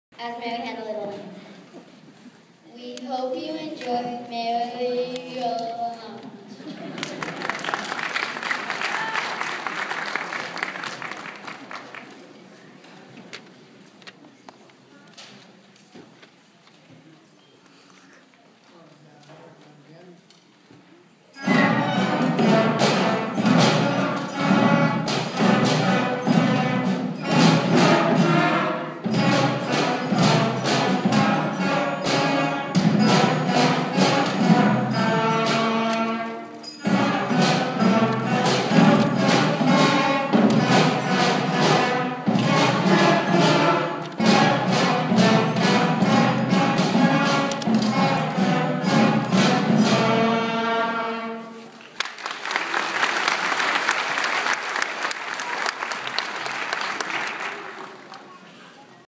Gina's beginner band debut #2
They played three melodies, I got 2 of them recorded.  This is the second one.